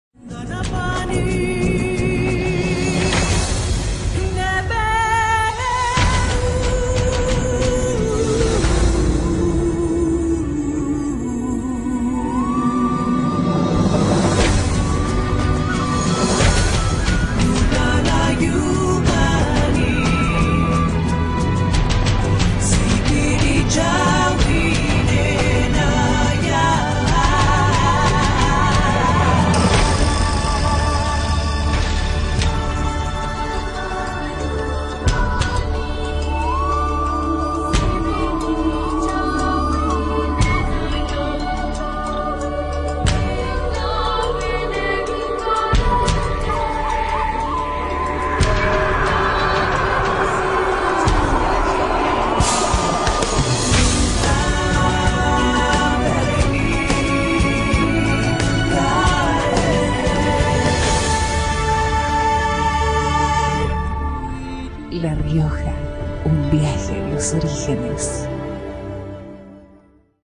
Monos con Alma - Reina Dpto Gral. Belgrano - La Rioja
Video de Presentación de La Reina del Dpto Gral. Belgrano realizada por "Monos con Alma - Producciones". Filmado en el Bosque junto al Dique de Olta. La Reina es Micaela Soledad Oliva